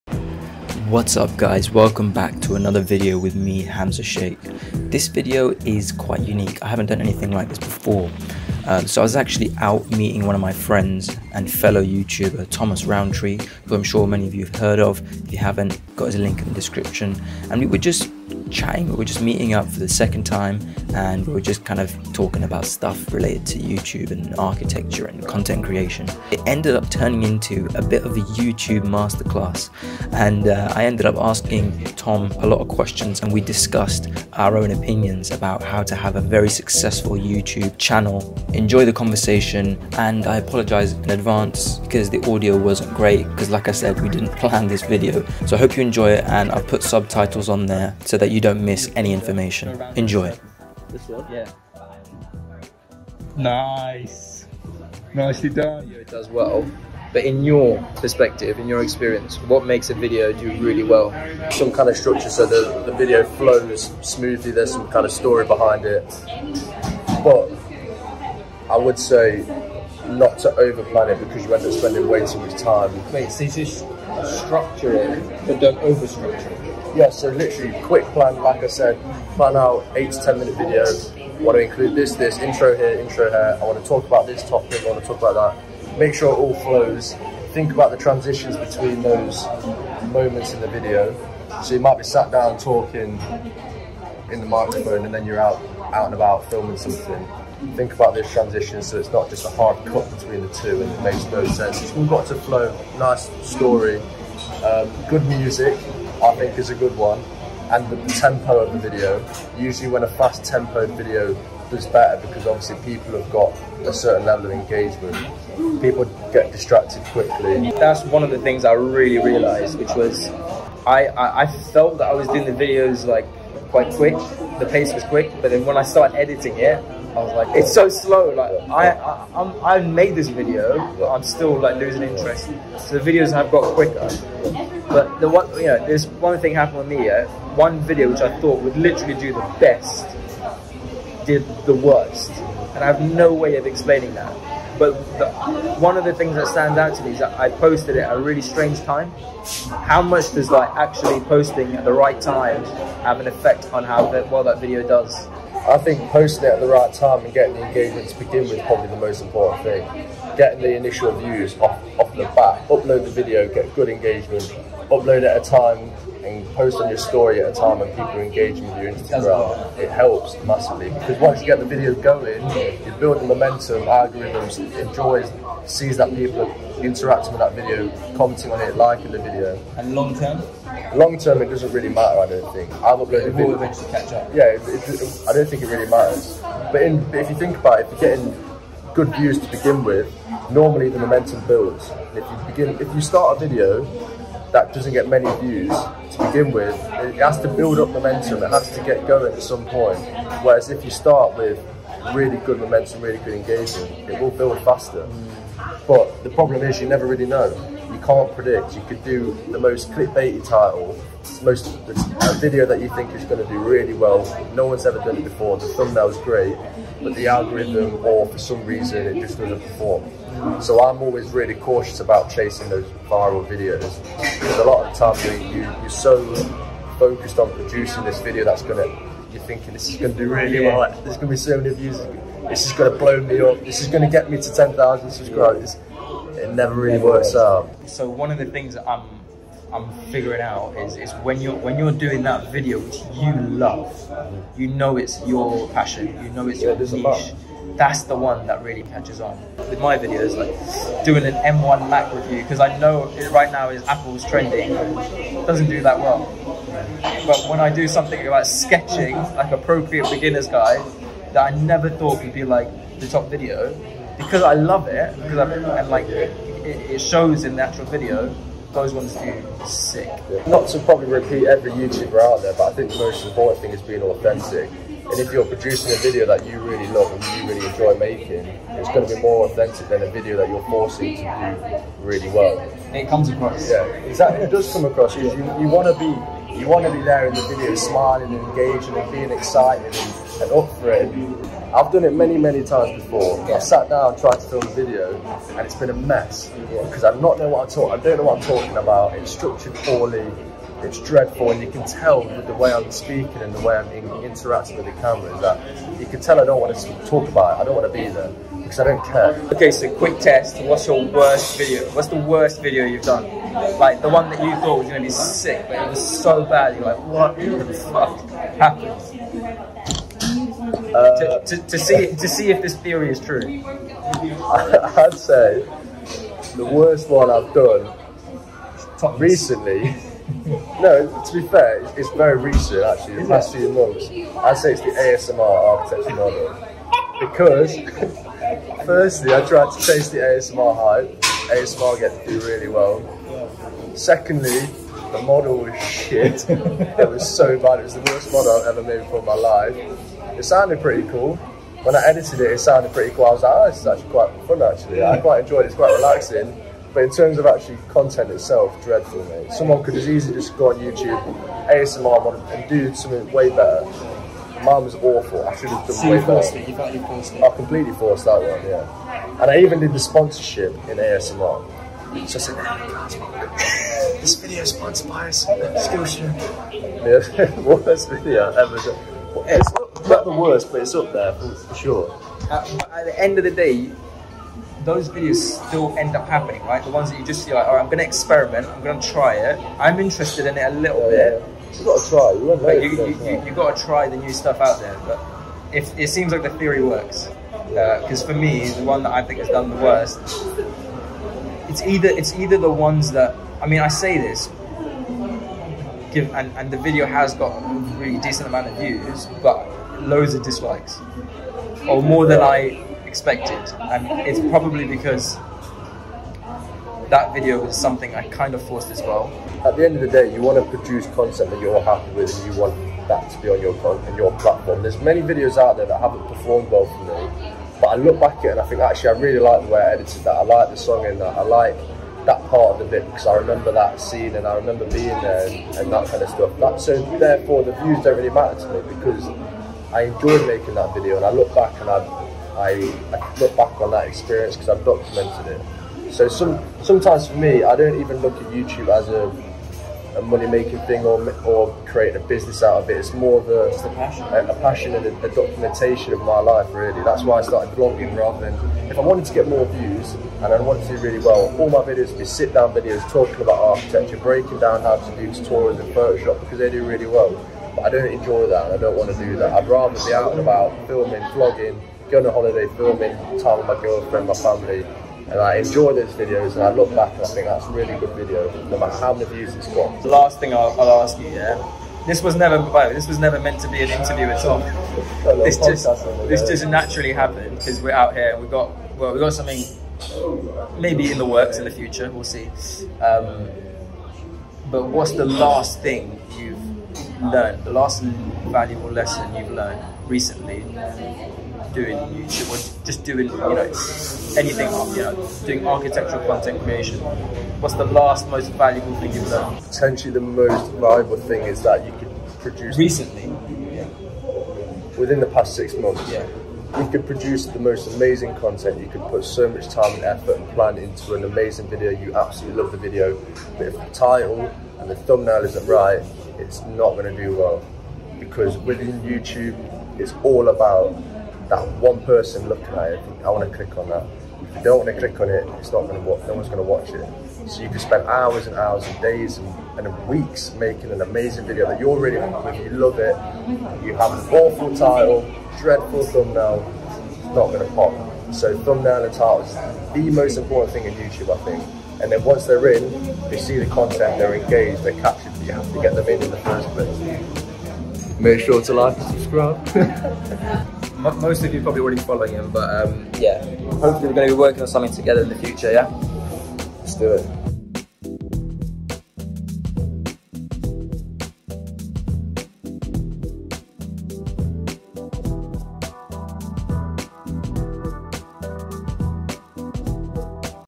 Beginner Tips for Youtube Success & Architecture Content Creation | with Thomas Rowntree
Here is some footage from my coffee shop conversation with fellow youtube and friend Thomas Rowntree. In this video, we talked about getting started and succeeding on youtube for architects and architecture students.

As content creators we just met to discuss some collaboration ideas and this conversation happened incidentally - I just got my phone out and recorded. We talked about high-level advice and tips for youtube beginners and those with a bit more experience.

Also, be sure to check out my architecture podcast series Two Worlds Design on my channel, or for audio,

You can also browse through all my architecture drawings on Instagram